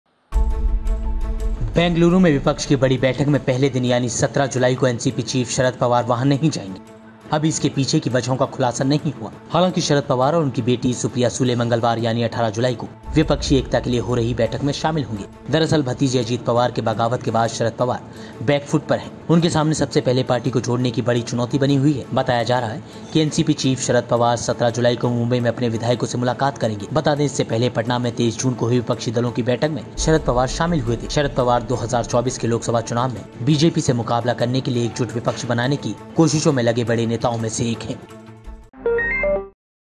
क्या विपक्ष को शरद पवार ने दिया झटका? 'विपक्षी एकता' की पहली बैठक से पिता और बेटी 'गायब'.
Mega Opposition Meet: कांग्रेस ने सोमवार को होने वाली यानि आज से कर्नाटक के बेंगलुरु में शुरू होने वाली दूसरी विपक्षी एकता बैठक के लिए कुल 25 पार्टियों से समर्थन जुटाया. NCP प्रमुख शरद पवार बेंगलुरु में संयुक्त विपक्ष की बैठक में भाग नहीं लेंगे.